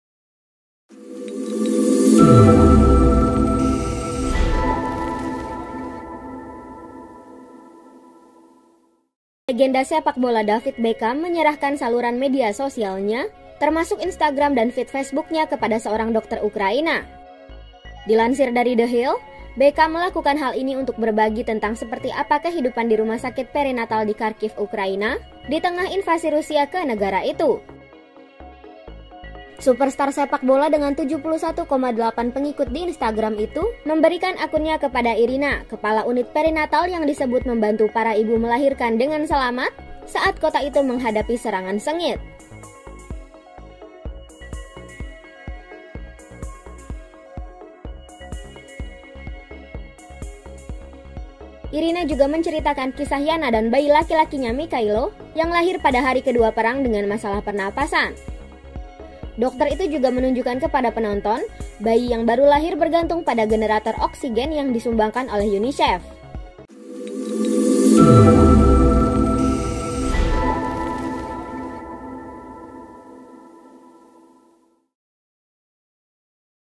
DAVID BECKHAM SERAHKAN AKUN INSTAGRAM KE DOKTER DI UKRAINA
Dilansir The Hill, Beckham melakukan hal ini untuk berbagi tentang seperti apa kehidupan di rumah sakit perinatal di Kharkiv, Ukraina, di tengah invasi Rusia ke negara itu.

Source : Kompas.com